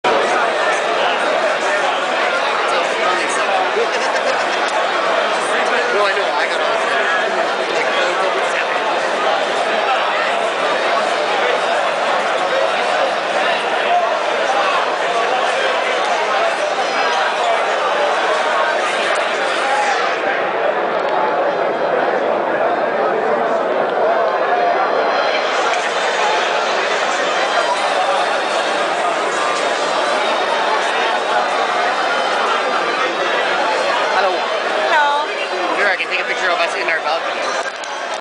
getting ready to run with the bulls 2011
Pamplona Spain, San Fermin Festival July 7, 2011. People getting ready to run with the bulls